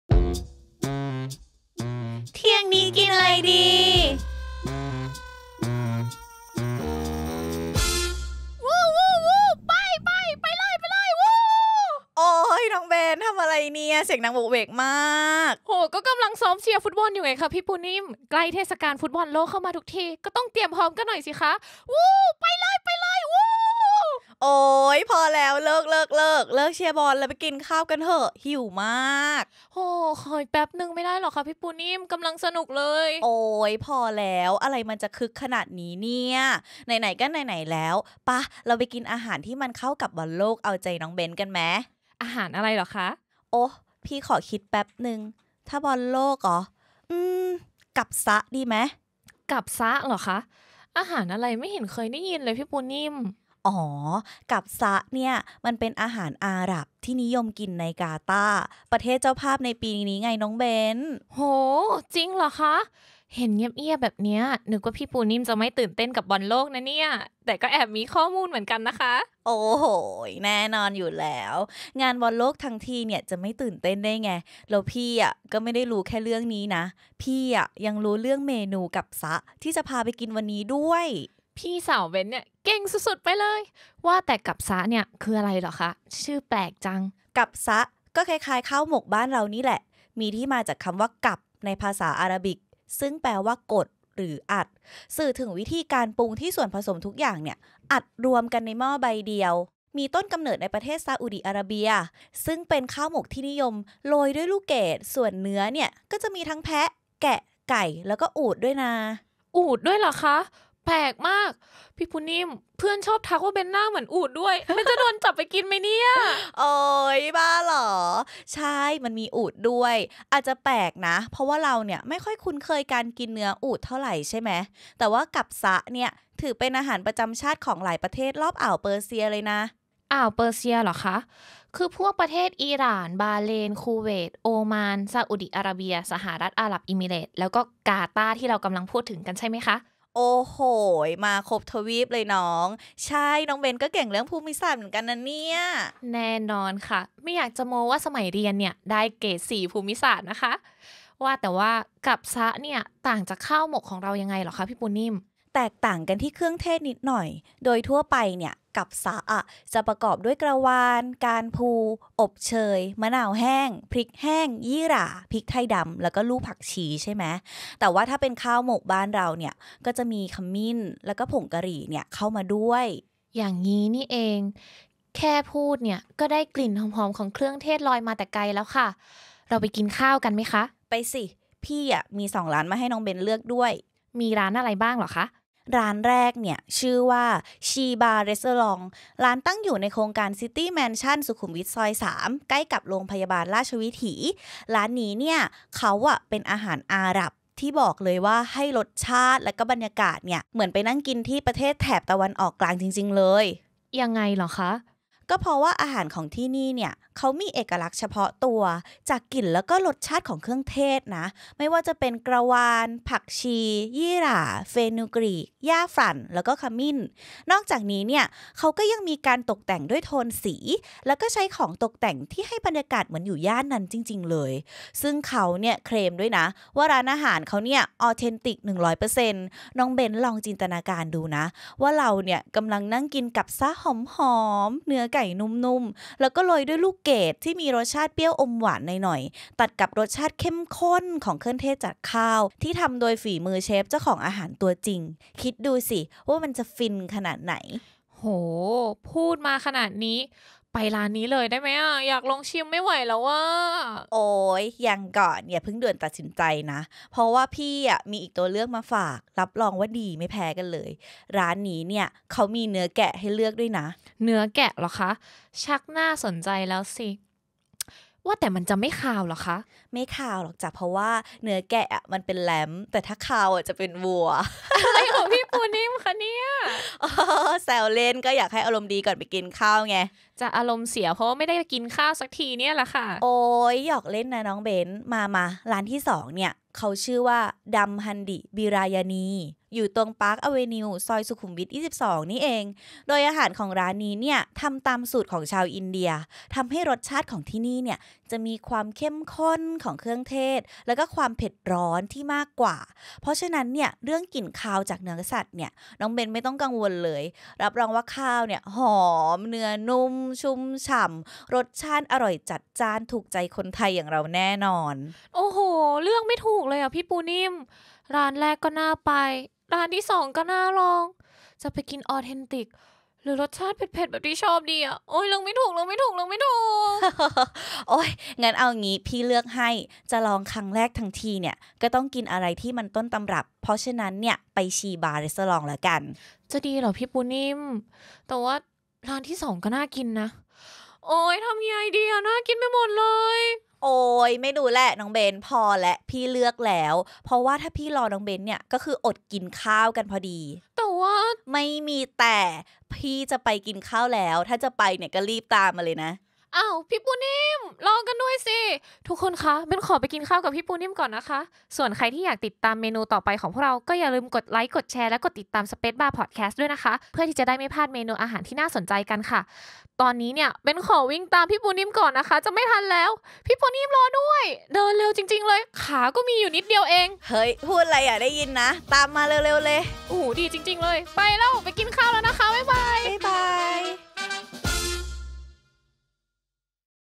เที่ยงนี้กินไรดี? : ‘กับซะฮ์’ ข้าวหมกละมุน หอมฟุ้งเครื่องเทศ
ปู๊น...ปู๊น ใกล้เทศกาลบอลโลกเข้ามาทุกที ‘เที่ยงนี้กินไรดี?’ มีเมนูมาฝาก!

‘เที่ยงนี้กินไรดี?’ ขอต้อนรับเทศกาลบอลโลกปี 2022 ด้วยการพาไปกิน ‘กับซะฮ์’ ข้าวหมกยอดฮิตจากประเทศเจ้าภาพอย่าง ‘กาตาร์’ ที่มีจุดเด่นอยู่ที่เครื่องเทศหอมๆ และเนื้อไก่ แพะ แกะ หรืออูฐนุ่มๆ อ่านไม่ผิด! มีเนื้ออูฐจริงๆ แต่เอ๊...ข้าวหมกกาตาร์ต่างจากข้าวหมกไก่บ้านเราอย่างไร?

ถ้าอยากรู้อย่าลืมติดตาม ‘เที่ยงนี้กินไรดี?’ เพราะเรื่องเชียร์บอลโลกเราก็ไม่ถอย เรื่องอร่อยเราก็ไม่ยอมแพ้เช่นกัน ส่วนเพื่อนๆ คนไหนที่มีร้านเด็ดร้านดังอยากแนะนำ ก็อย่าลืมมาร่วมแบ่งปันแชร์ร้านกันได้ที่ช่องคอมเมนต์ด้านล่างเลย👇

ฟัง PODCAST: เที่ยงนี้กินไรดี? ฟังพร้อมกันmทุกวันพุธ เวลา 09.30 น. ทาง

ติดตามเรื่องราวที่ผู้คนสนใจได้ทุกวันที่